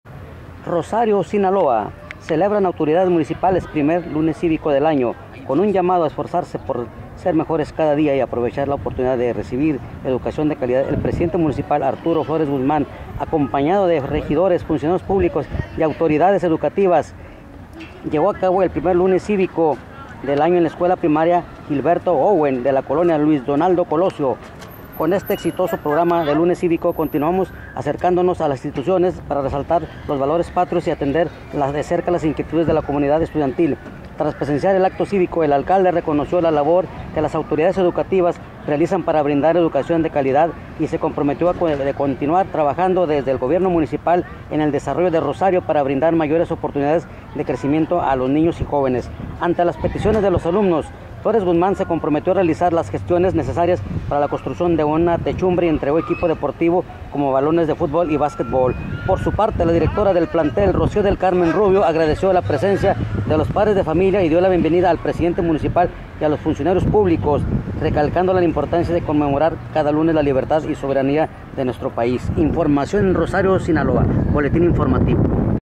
Rosario,Sinaloa, autoridades mpales.celebran 1er.lunes civico del año
ROSARIO,SINALOA